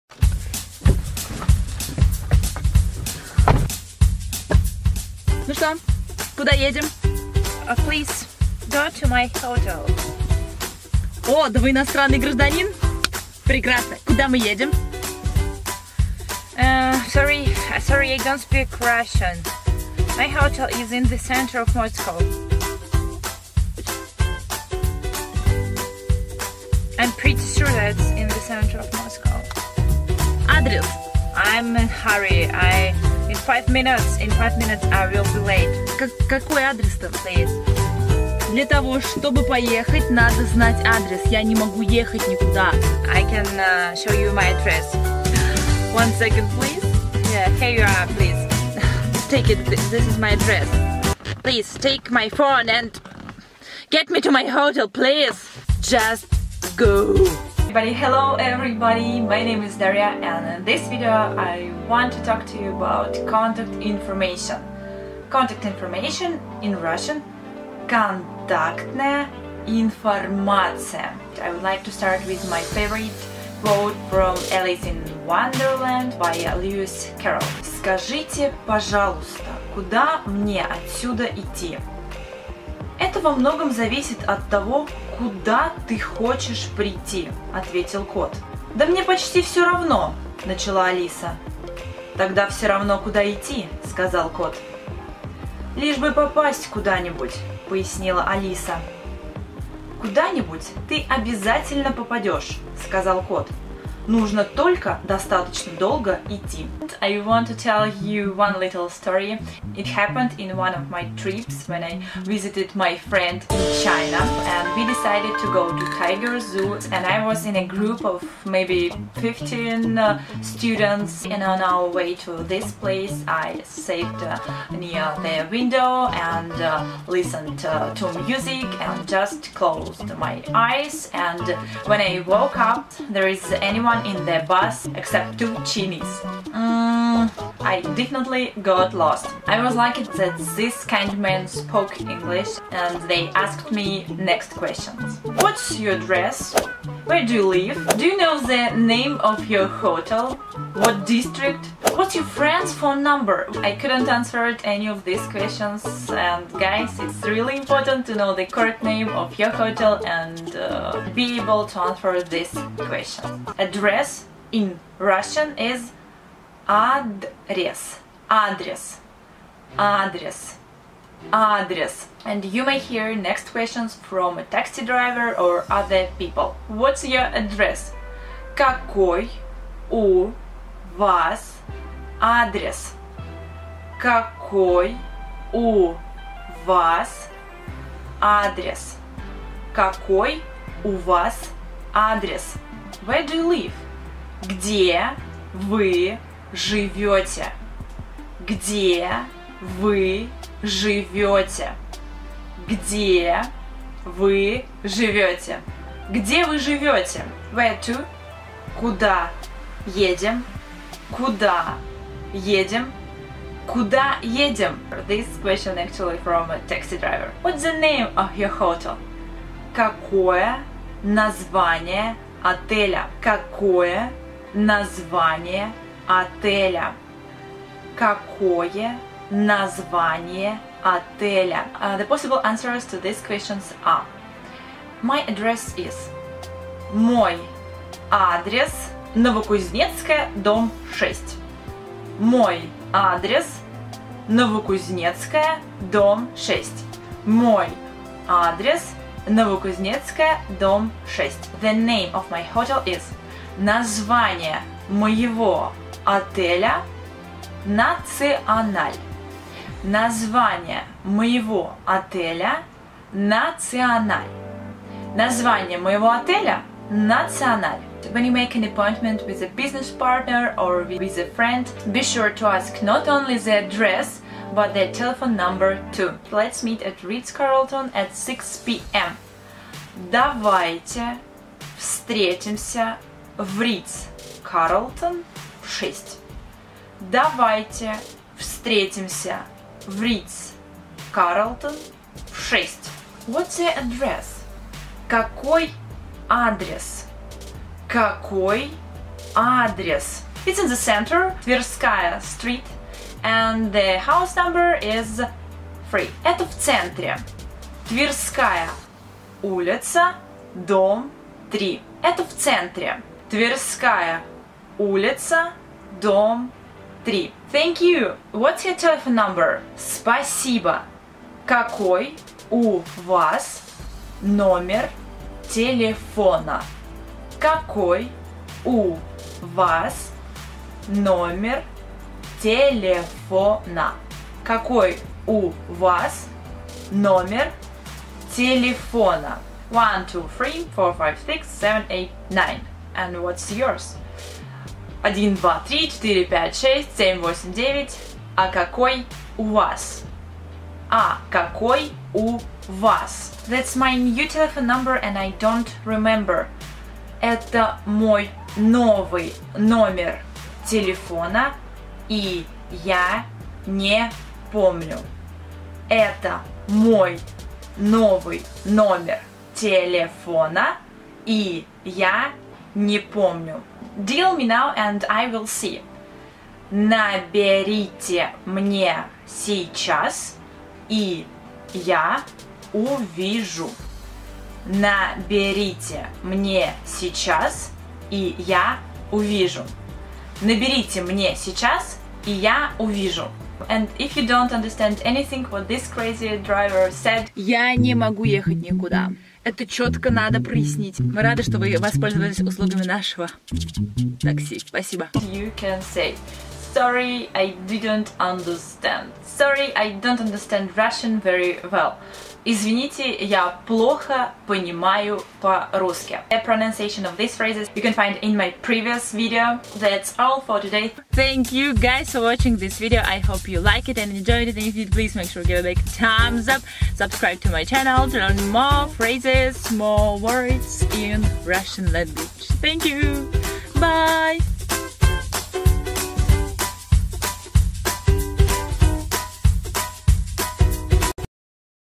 Address in Russian. How to ask and reply | Learn Russian
Hello everybody! My name is Daria.
Address in Russian – адрес
You may hear next questions from a taxi driver or other people.
What’s your address? Какой у вас адрес?
Where do you live? Где вы живете?
Where to? Куда едем?
What’s the name of your hotel? Какое название отеля?

My address is … Мой адрес…
The name of my hotel is – Название моего отеля…

When you make an appointment with your business partner or a friend, be sure to ask not only the address but a telephone number too.

If you don’t understand anything use these phrases
Sorry, I didn’t understand – Извините, я вас не понимаю
Sorry, I don’t understand Russian very well – Извините, я очень плохо понимаю по-русски